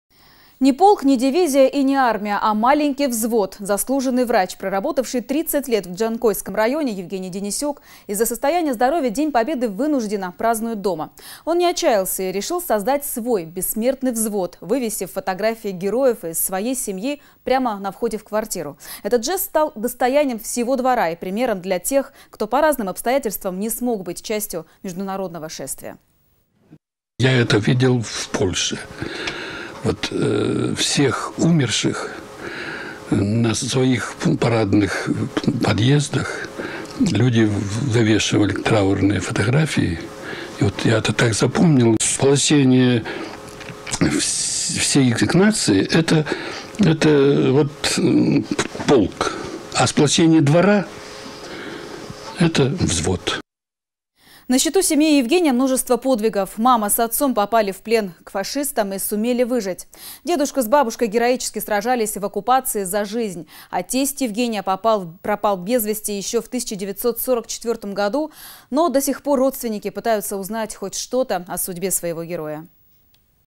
Не полк, не дивизия и не армия, а маленький взвод
Заслуженный врач, проработавший 30 лет в Джанкойском районе, Евгений Денесюк из-за состояния здоровья вынужденно празднует день победы дома. Он не отчаялся и решил создать свой бессмертный взвод, вывесив фотографии героев из своей семьи прямо на входе в квартиру. Этот жест стал достоянием всего двора и примером для тех, кто также по разным обстоятельствам не смог быть частью международного шествия.